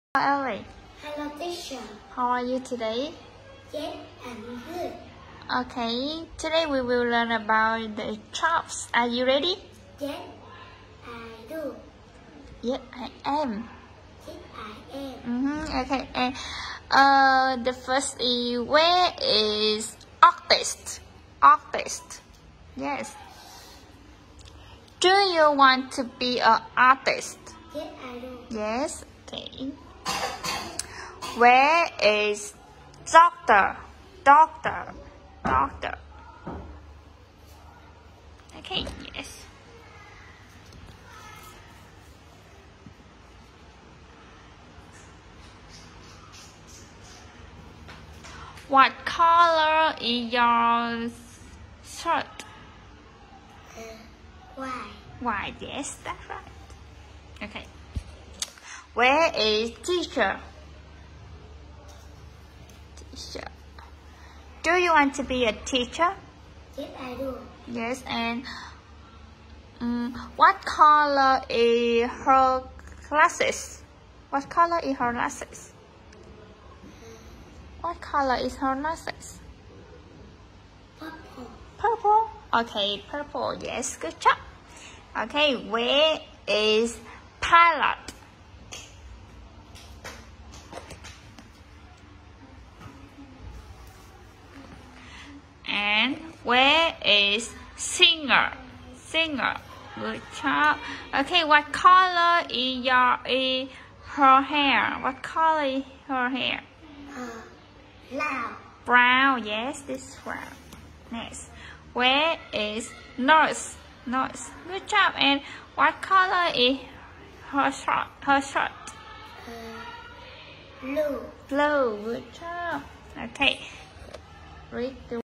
Tiếng Anh Giao Tiếp Thiếu Nhi Tại Phan Thiết: Elly talking about Jobs
Elly practices talking about Jobs by answering and asking questions with Rosie!